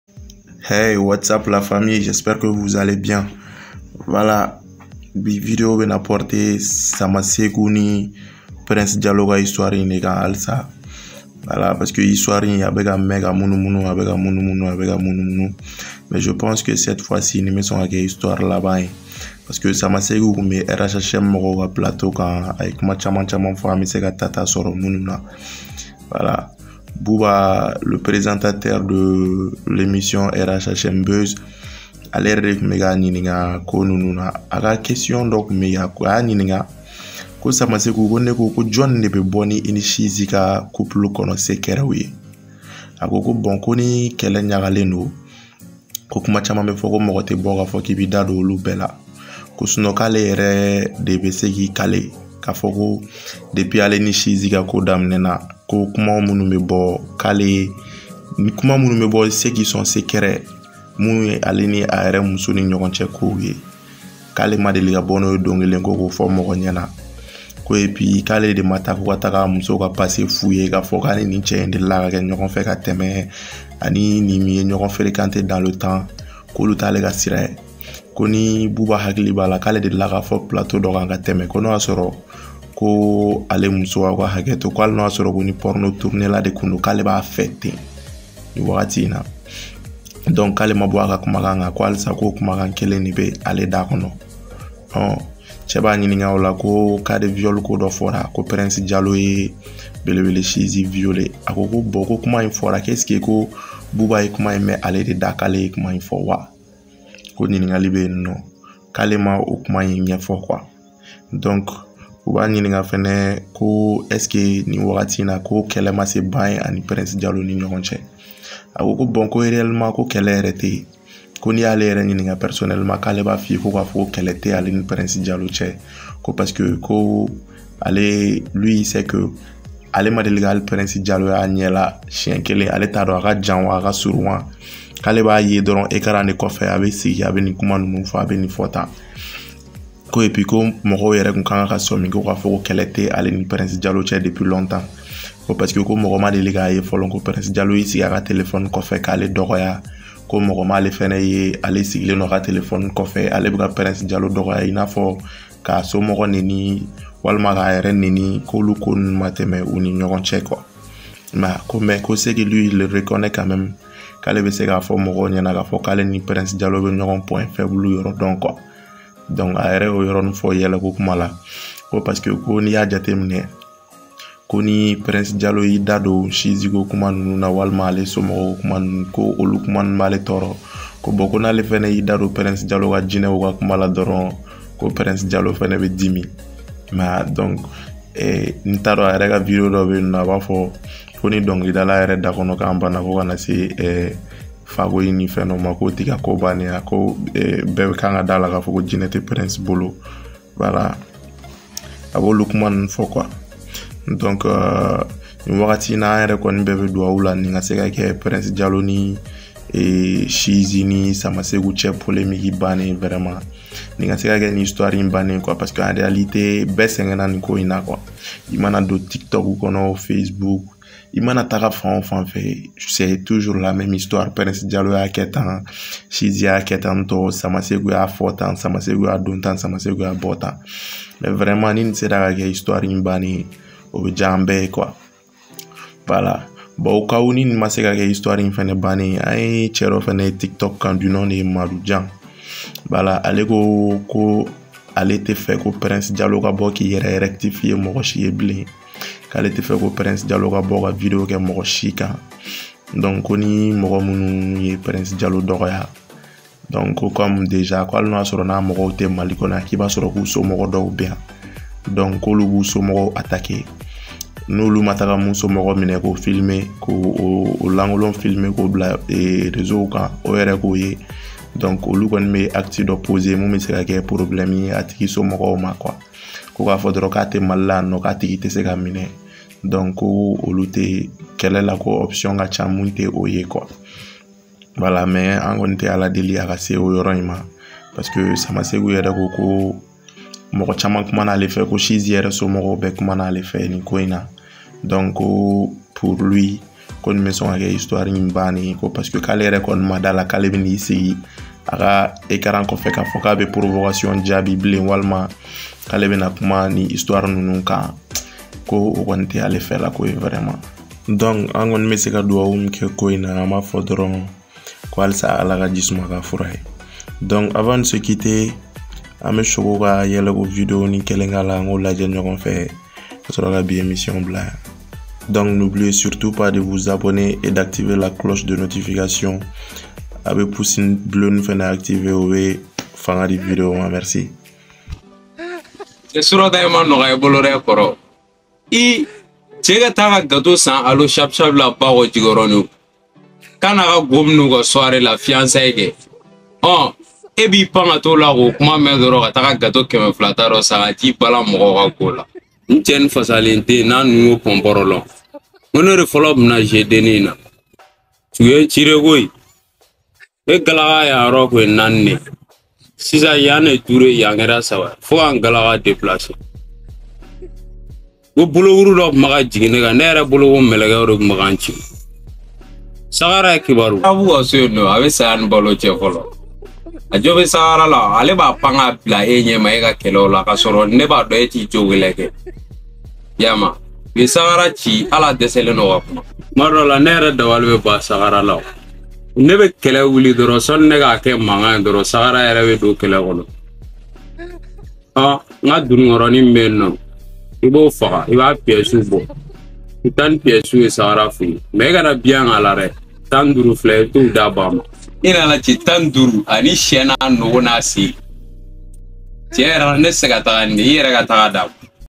SUITE ET FIN DE L'HISTOIRE ENTRE PRINCE DIALLO - SAMASSEKOU ET BELEBELE CHIZY
Dans cette vidéo il sera question de la polémique entre prince diallo- mohamed samassekou GD et belebele chizy